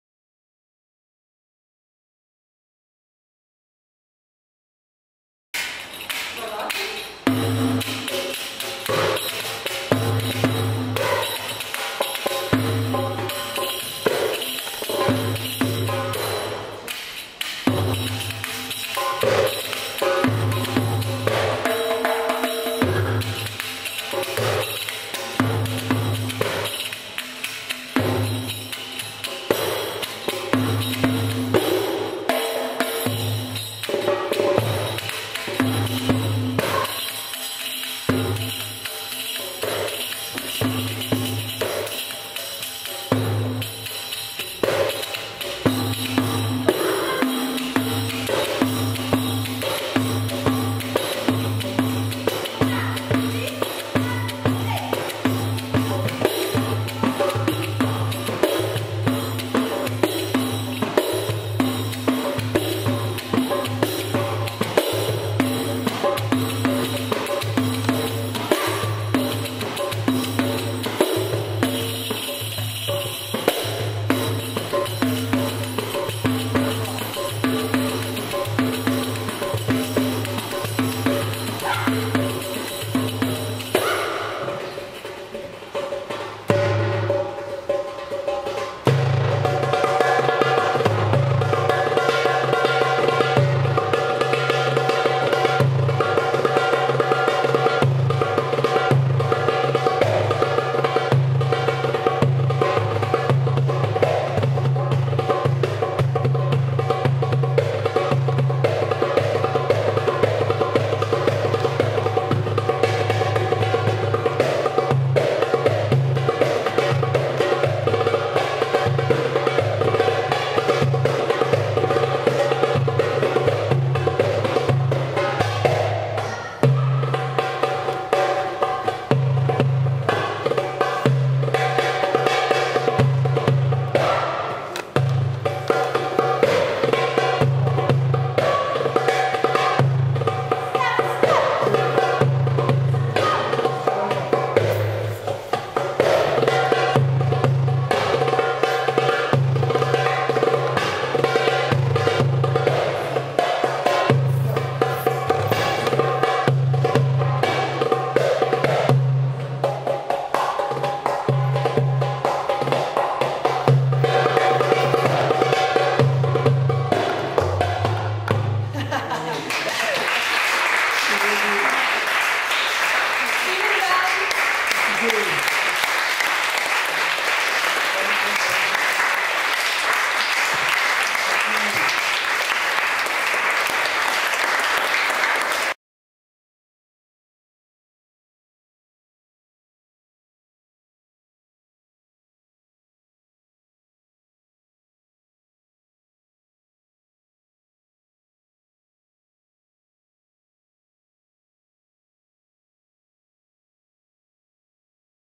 Summerstage 2013 - Work in Progress of Contemporary Oriental Dance - Mirjam Sutter
Final Showing of one week dance work.
Live Percussion from Kamal and Taoufiq Essahbi
Dance Teaching by Mirjam Sutter